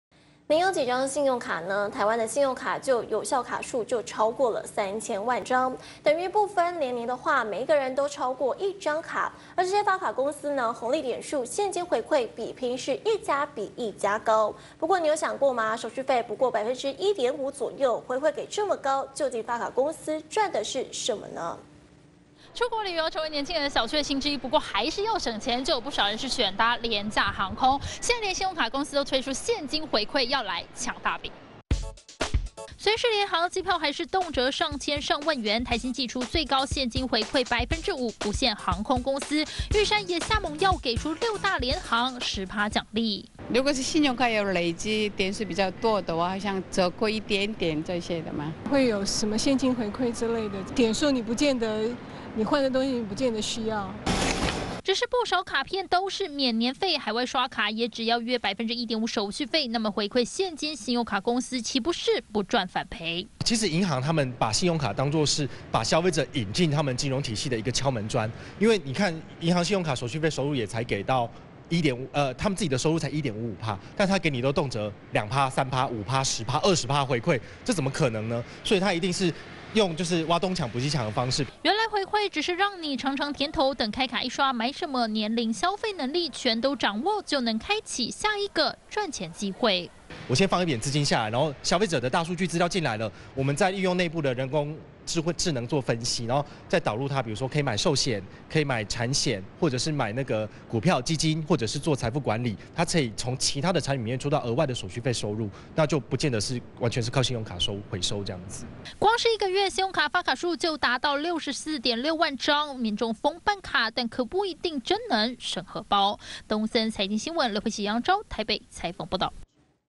現金回饋衝上10%！ 揭密信用卡公司賺什麼
信用卡廣告，都打著現金回饋，告訴你，快點刷，紅利點數、現金回饋，一家比一家給的還要兇，但裡面有秘密的！您有想過嗎？手續費才1.5%左右，回饋給到2%、3%這麼高？發卡公司賺什麼呢？原來她要透過大數據，拿你的資料。

● 鎖定【東森亞洲新聞】 周一至週五18:00 ~ 20:00